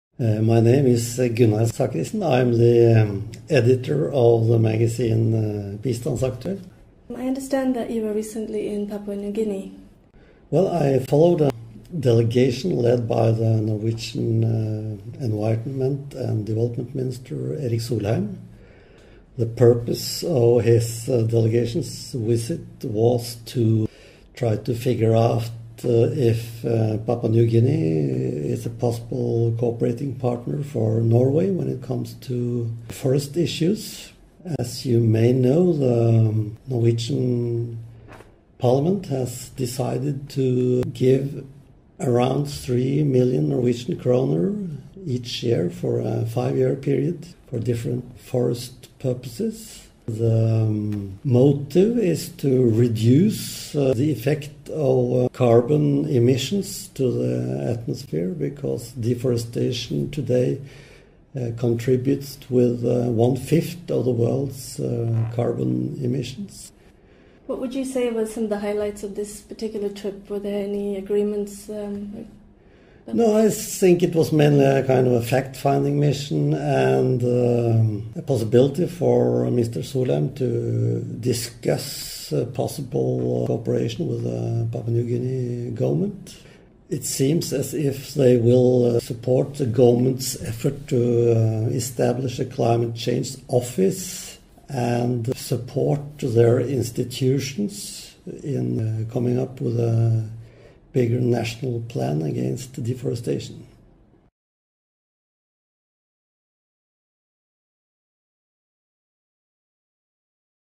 Solheim lover skogpenger
Om reisen til Erik Solheim til Papua Ny Guinea fortalt av Bistandsaktuelts redaktør Gunnar Zachrisen.